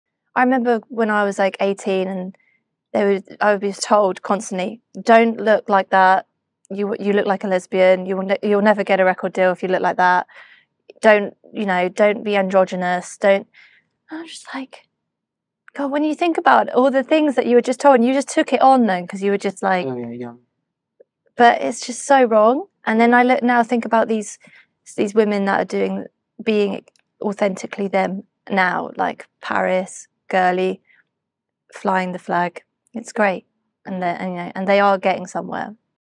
"Don't dress like that" Jessica Winter & Tom Rasmussen | Duets
As part of our Pride month Duets series, Jessica Winter tells Tom Rasmussen how outdated gender stereotypes impacted her early life as an artist.